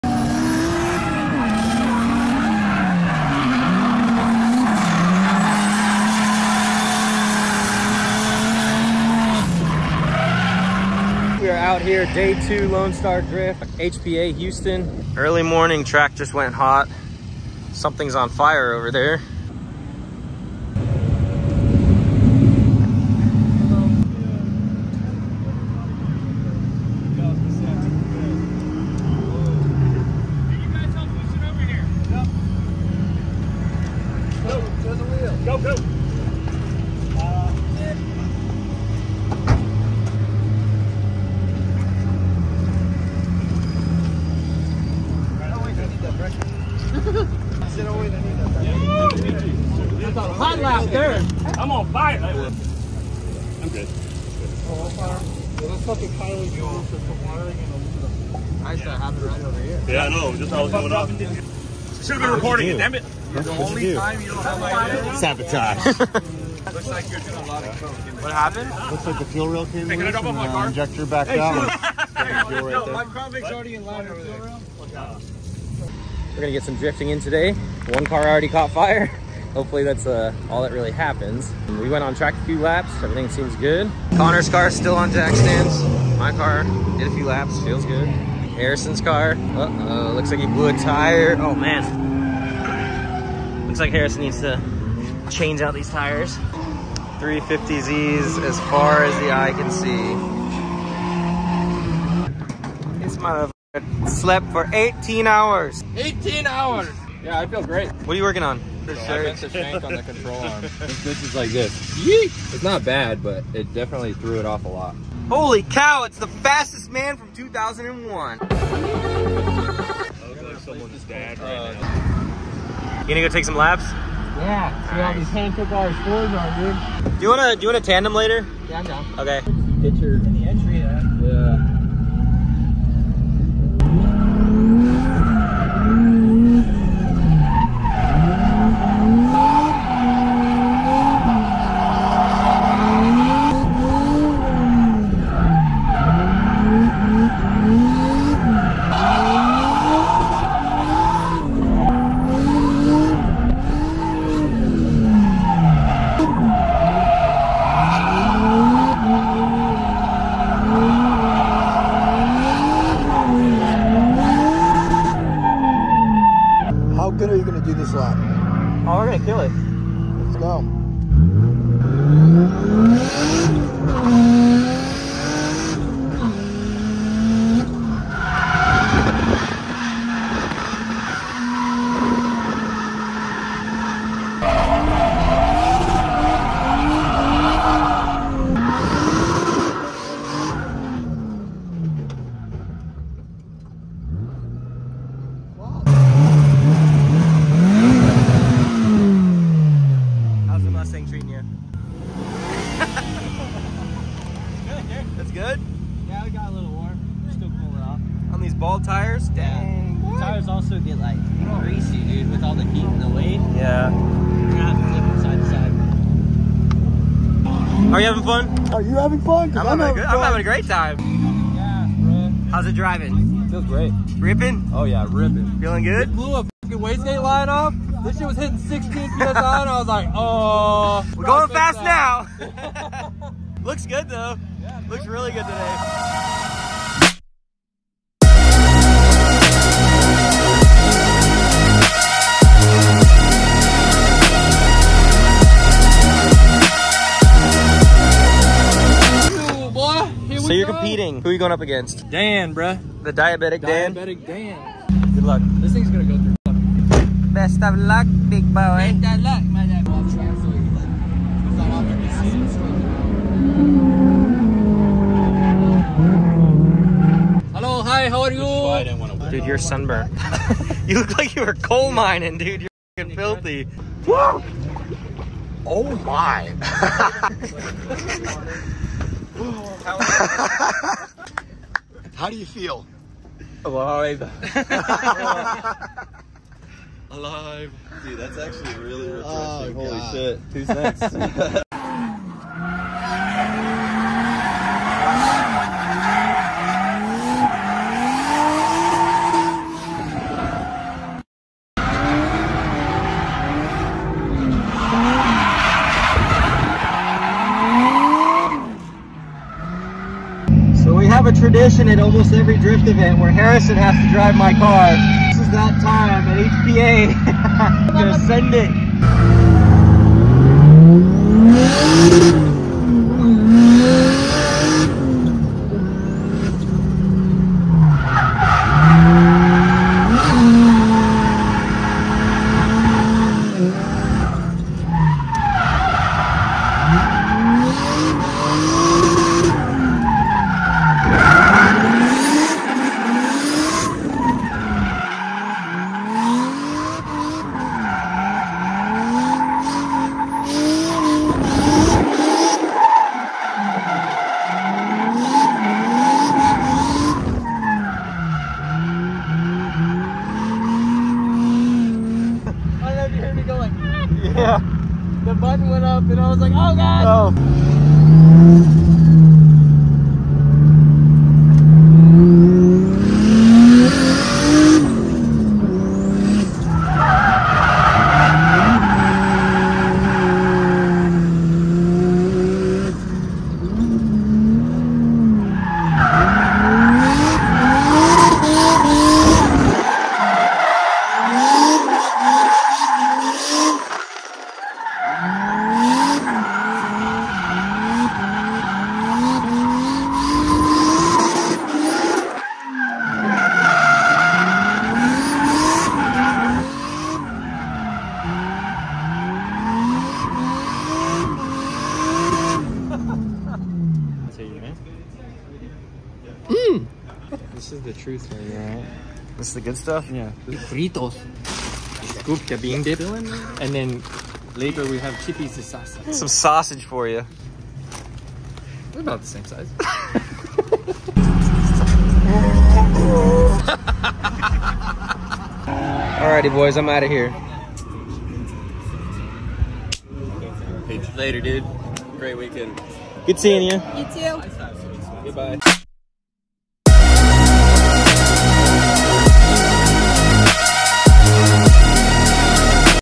DRIFTING my TURBO IS300! | CAR caught on FIRE
I took my IS300 drifting at the Houston Police Academy for@LoneStarDrift

Go check out @harrisondrifter

Thanks for the likes and subs :D
Don't forget to check out the RED OR DEAD store

Thanks to @LifeOfACyborg for the first clip!